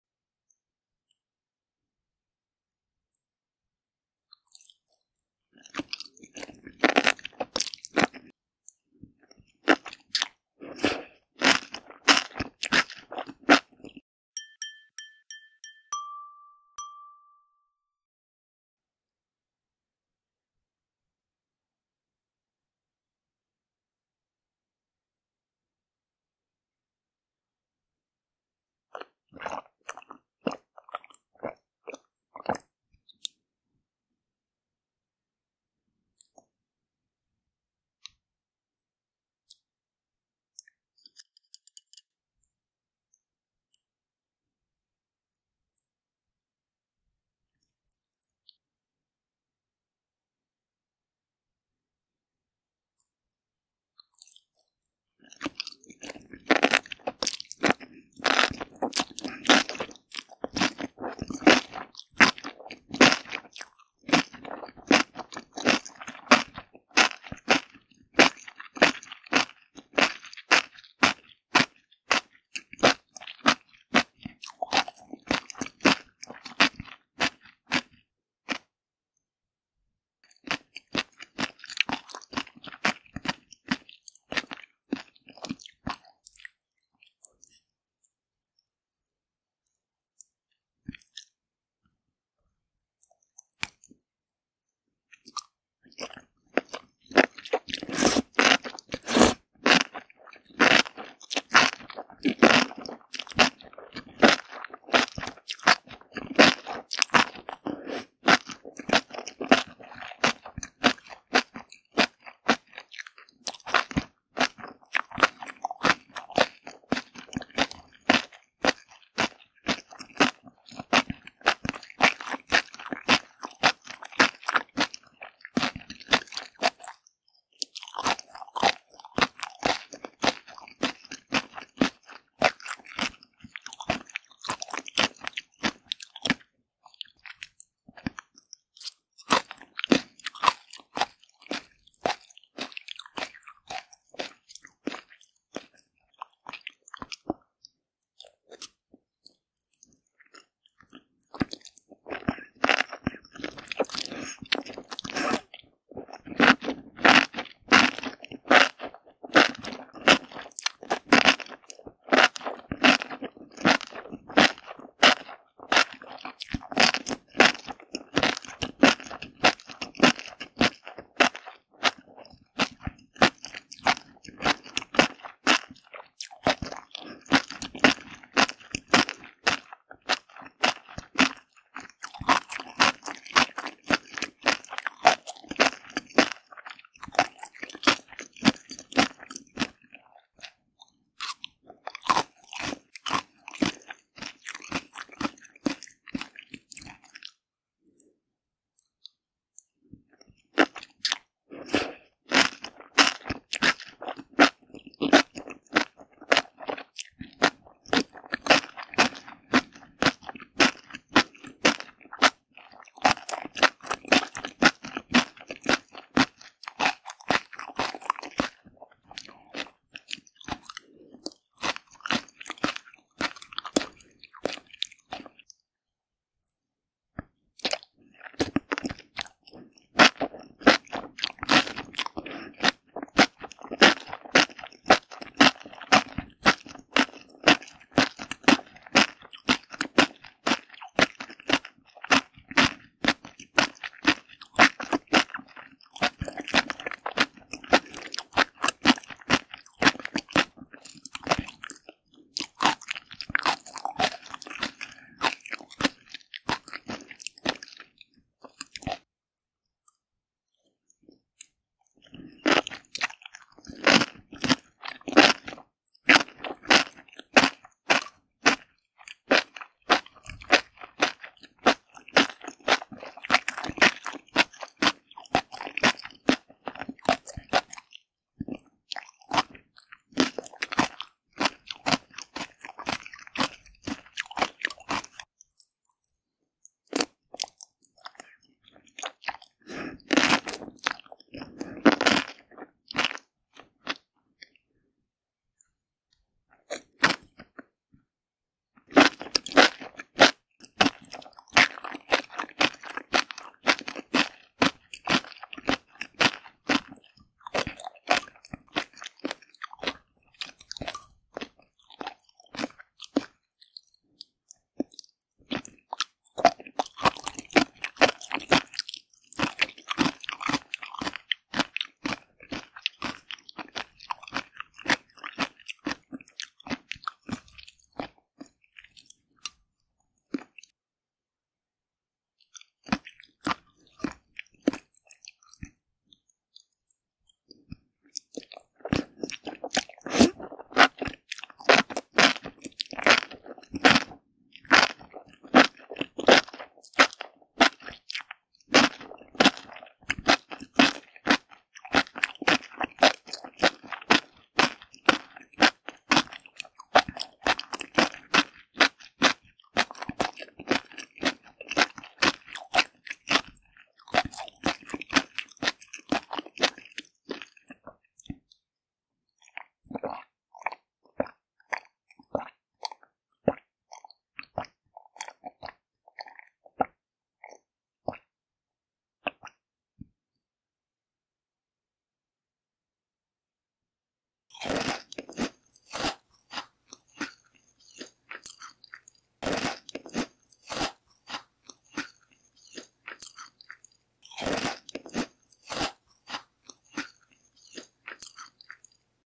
SUB) ASMR SPICY FIRE SAUCE ENOKI MUSHROOMS, SUPER BIG BITES, CRUNCHY, MUKBANG 먹방, REAL EATING SOUNDS
*ASMR SPICY FIRE SAUCE ENOKI MUSHROOMS, CRUNCHY, SUPER BIG BITES.
*MUKBANG, 먹방, 묵방, KOREAN, NO TALKING, ASMR, EATING SOUNDS, EATING SHOW.